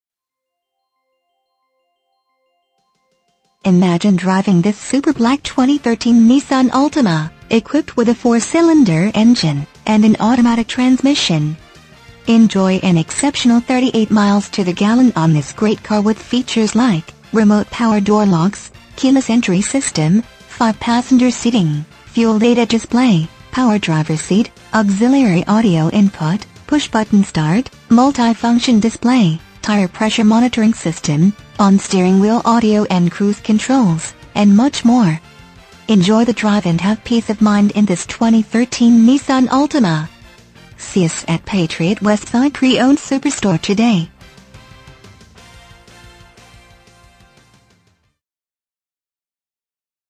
2013 Nissan Altima Patriot Westside Pre-Owned Superstore Evansville, IN 47712
Nissan Altima Check out this Super Black Nissan Altima 4 Dr Sedan, equipped with a 4 Cylinder engine and an automatic transmission. Enjoy an exceptional 38 miles to the gallon on this great car with features like Remote Power Door Locks, Keyless Entry System, 5 Passenger Seating, Fuel Data Display, Power Driver's Seat, Wheel Covers, Auxiliary Audio Input, Push Button Start, Multifunction Display, Tire Pressure Monitoring System, on steering wheel audio and cruise controls, and much more. Enjoy the drive and have peace of mind in this Nissan Altima. See us at Patriot Westside Pre-Owned Superstore today!
MILES: 51,433
VIN:1N4AL3AP4DC236167

Patriot Westside Pre-Owned Superstore
110 S. Rosenberger Ave.